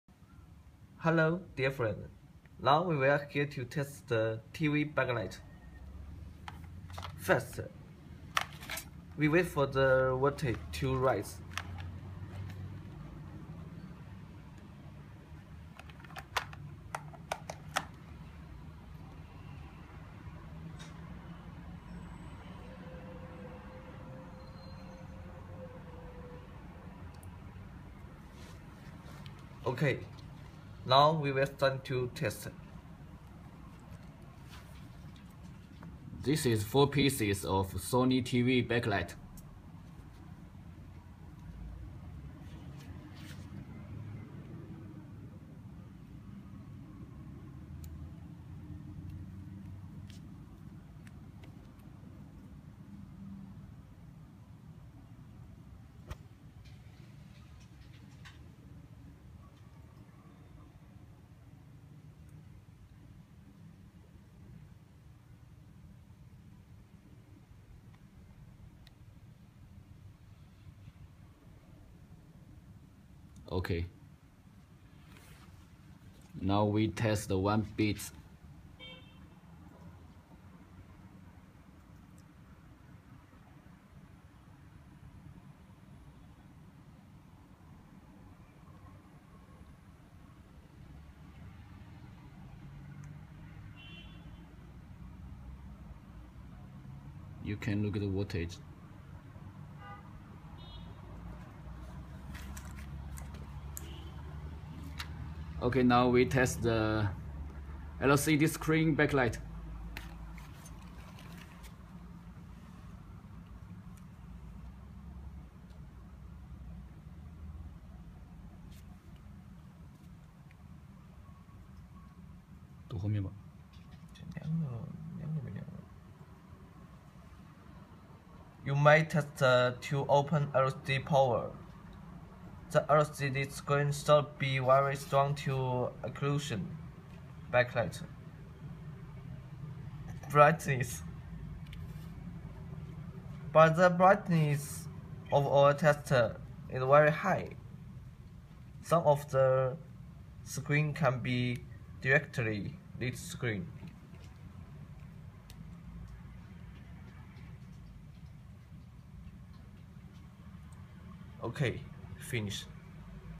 LED Back light Tester for All LED TVs
You can use this tool for testing all type of Back light strips..

Led lcd tv backlight tester tool for tv repair from